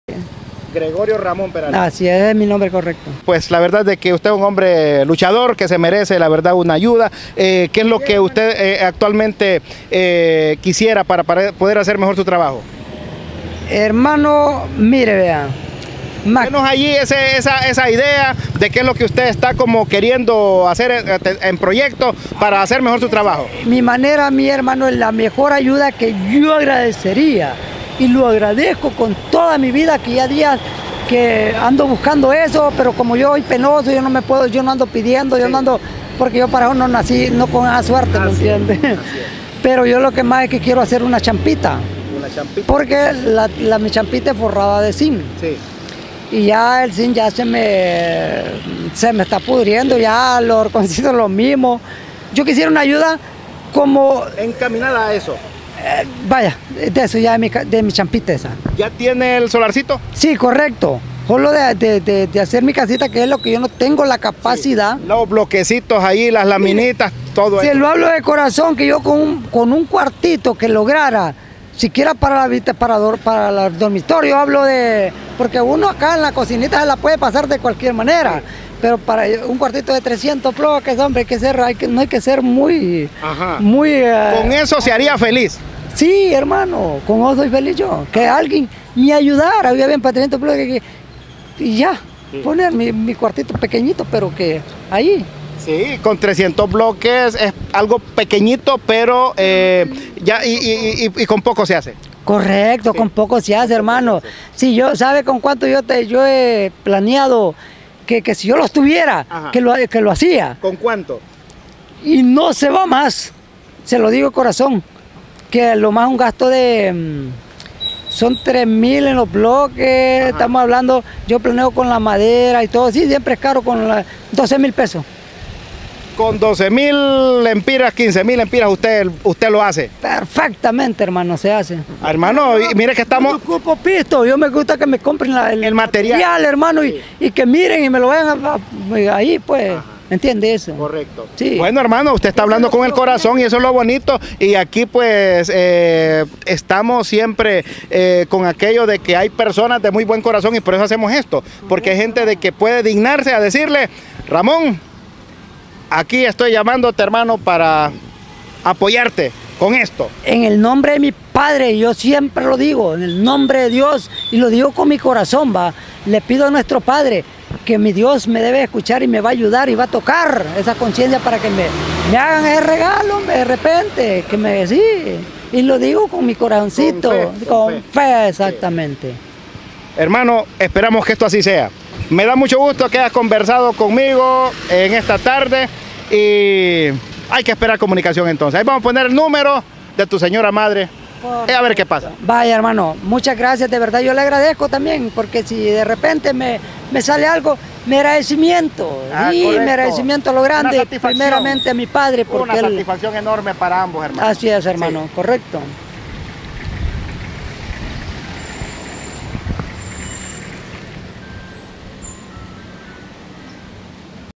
El Buki original y lo que más anhela de corazón
El Buki original con particular honestidad nos ha dicho Lo que más anhela desde su corazón
gracias por mirar el vídeo